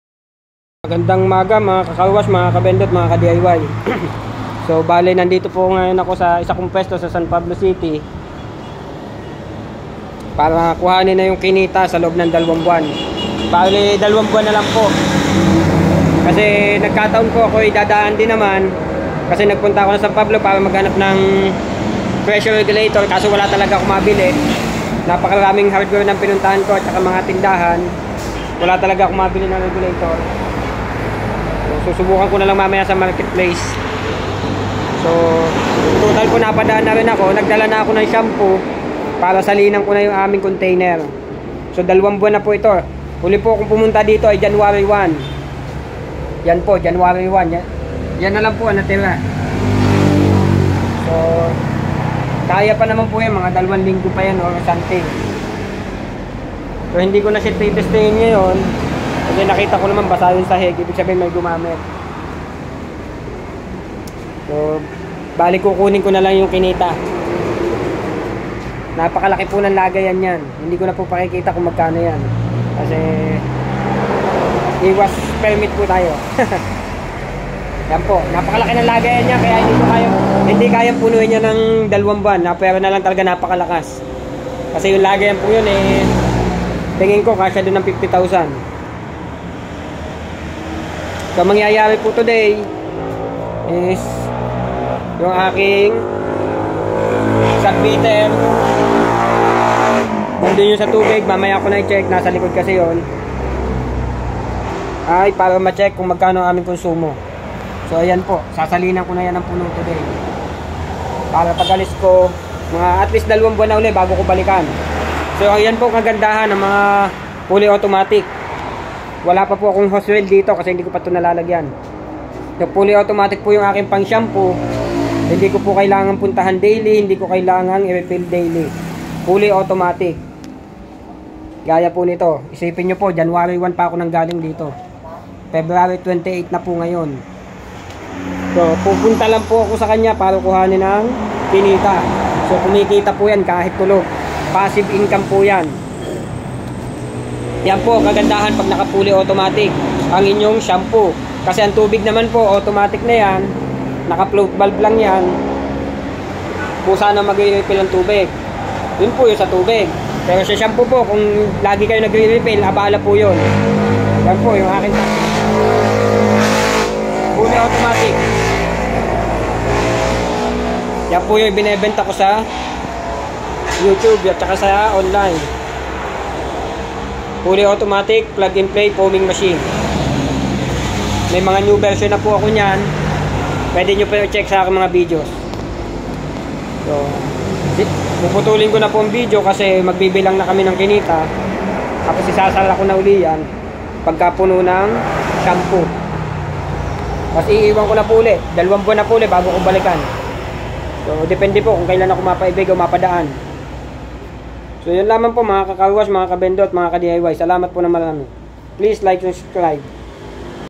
2 months bago balikan, Tunay n passive income. Carwash with Fully Automatic Foaming Machine
Located @ Brgy. Balanga San Pablo City.